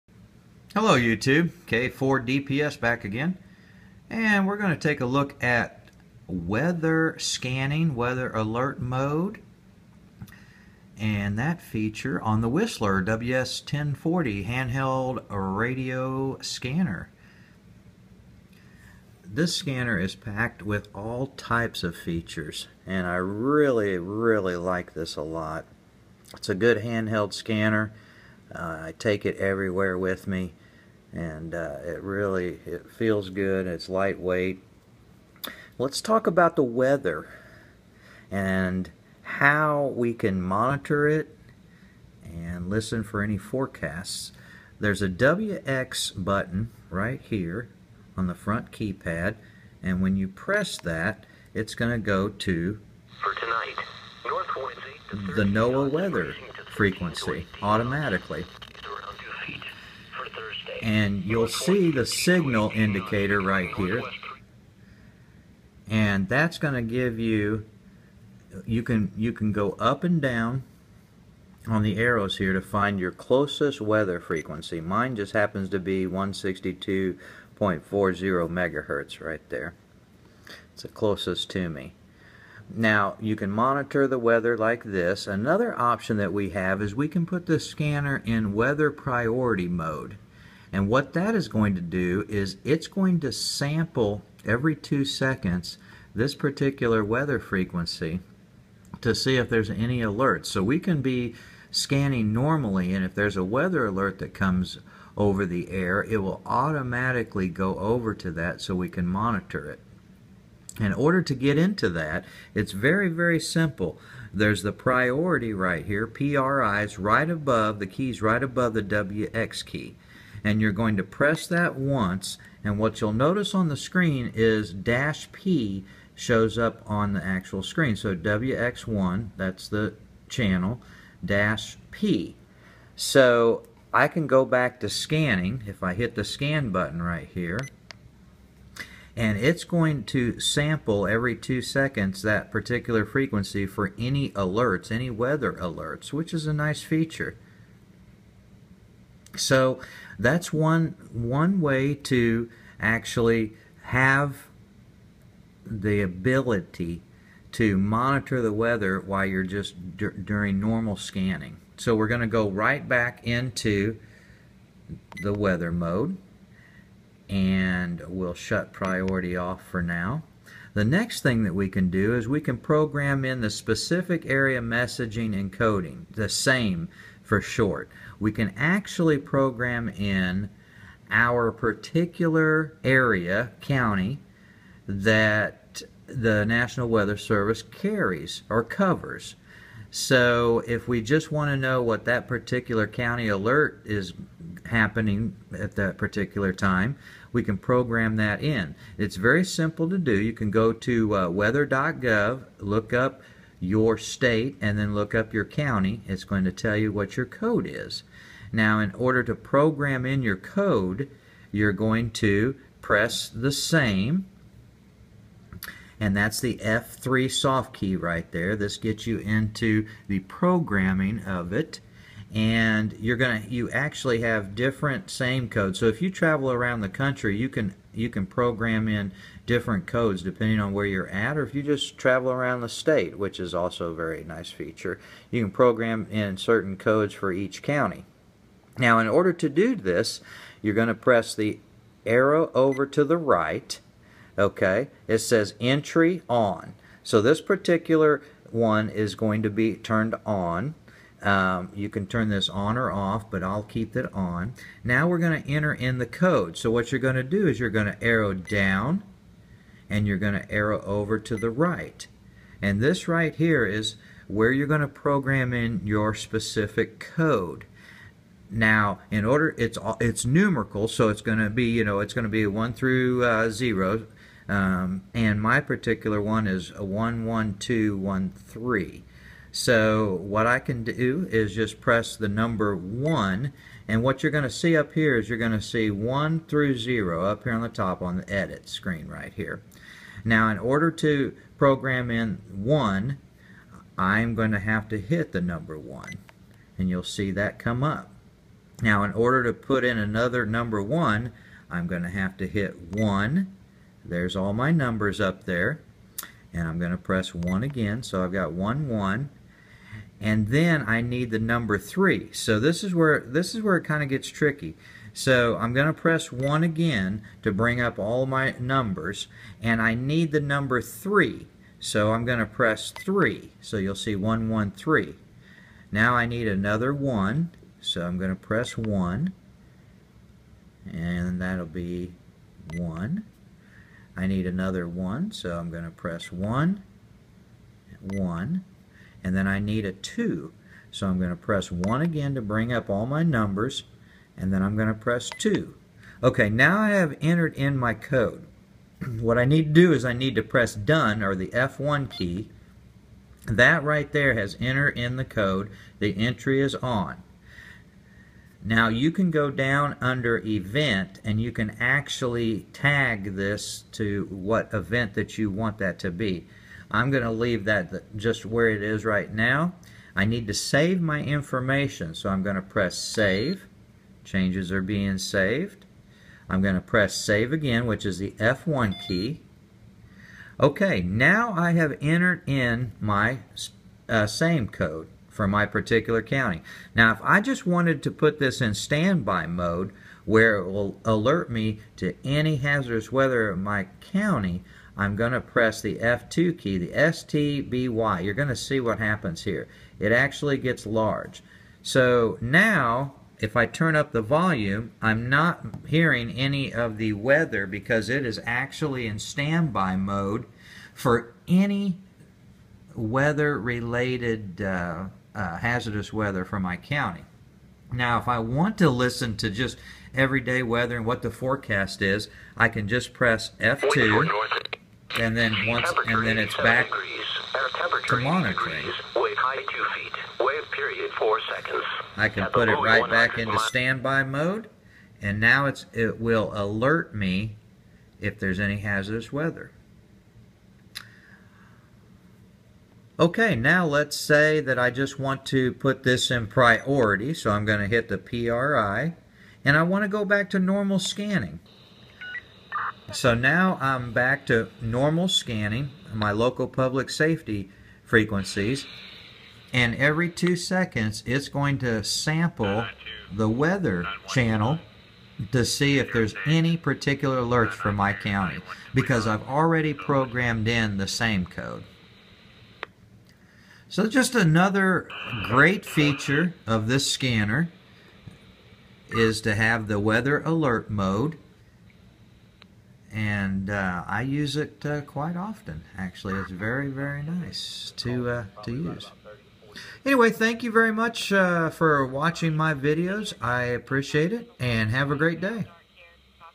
Whistler WS1040 Weather Scanning and SAME programming.
A look at weather scanning, weather priority mode, and SAME code programming for the Whistler WS1040 handheld digital radio scanner.